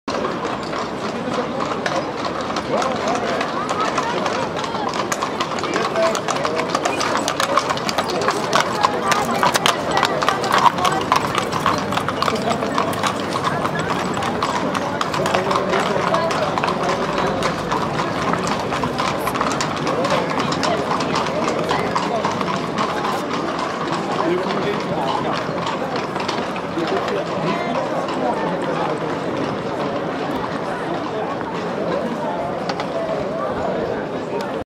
Horse Drawn Carriages Vienna Austria
Horse drawn carriages in Vienna Germany.  Filmed with Canon Vixia HG20 camera.